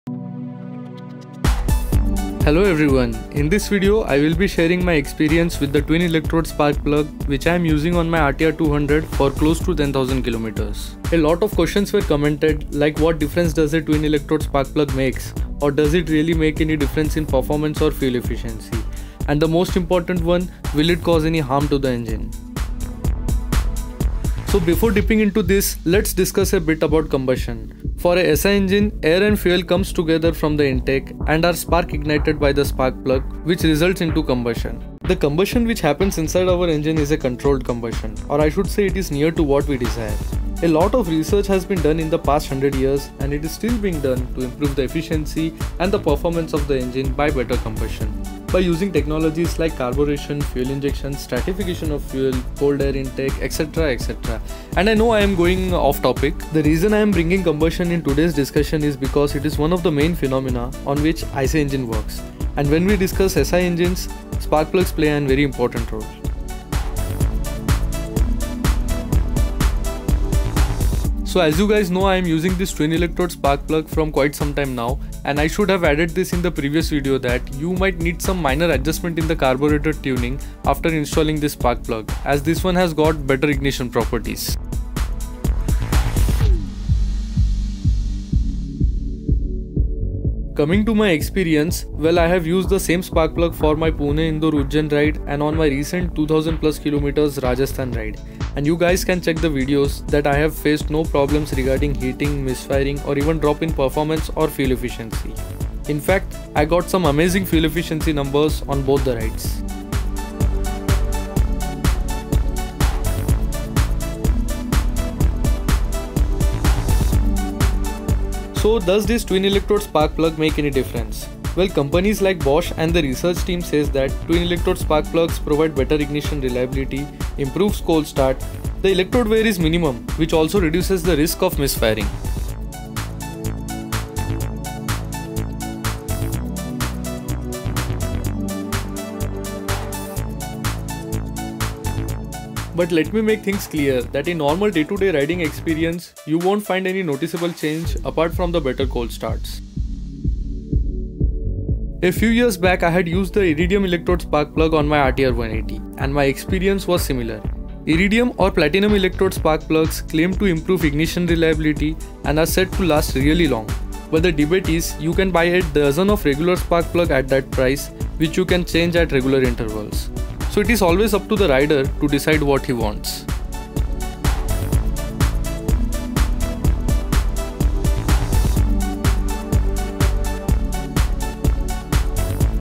TWIN ELECTRODE SPARKPLUG REVIEW, BEST SPARK PLUG? IRIDIUM PLATINUM SPARKPLUG EXPERIENCE | TVS APACHE
Bosch Twin electrode spark plug

This video is an update to the twin electrode sparkplug setup which I am using in my RTR 200. Almost 10000km are done with this setup & a lot of questions are asked about it so here is a update video & I have also discussed about my past experience with Iridium electrode sparkplug.